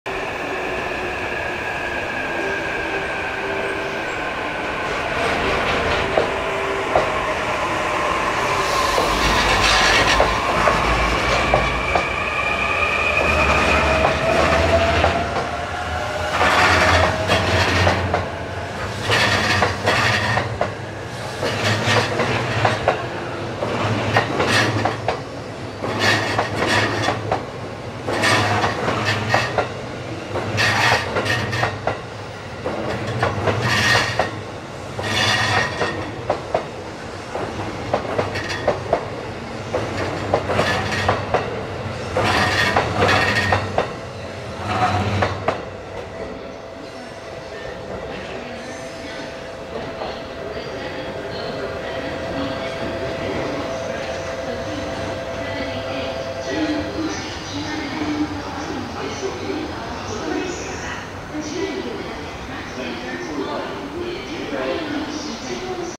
★20240512 E531×E531系 発車 ＠上野
撮影路線：JR東日本 宇都宮線（上野東京ライン）
撮影駅：上野
行先：普通 品川
車両：E531×E531系
両数：10+5両＝15両
MH：ーー
備考欄：